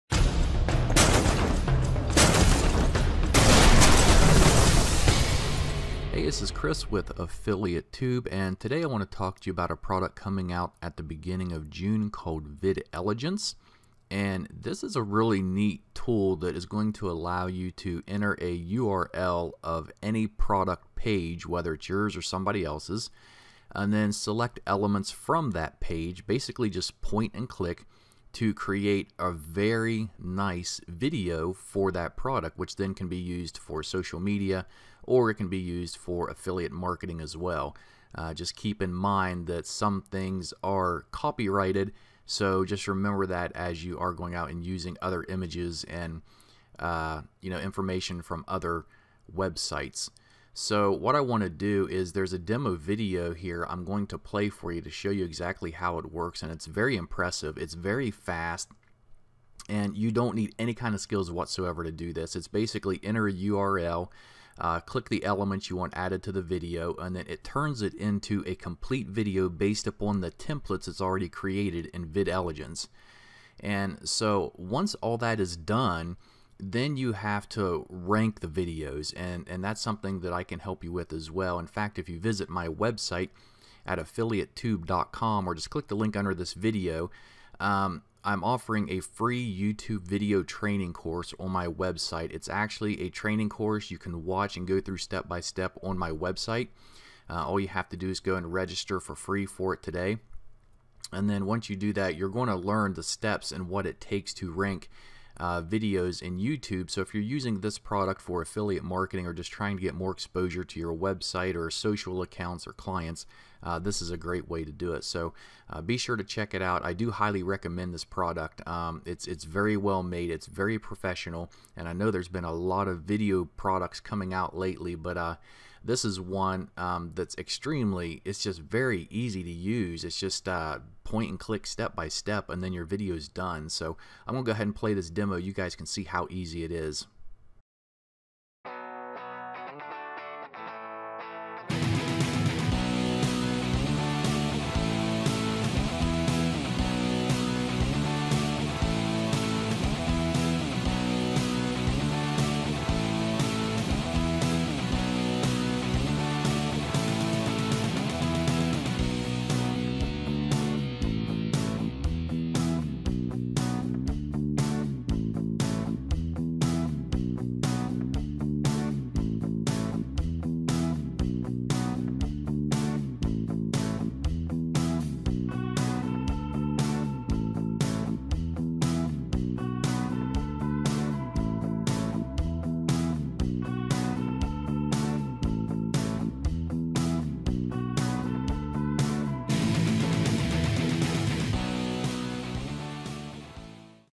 Videlligence Review - Videlligence Demo and Bonus
Videlligence Review - Videlligence is a cloud-based tool that allows you to enter the URL of any website and create a video from it almost hands-free.  Once you enter a URL of a website, Videlligence will import the content, images and pricing which you then can select to use as part of the video.

More on:

videlligence
videlligence review
videlligence reviews
buy videlligence
videlligence discount
videlligence demo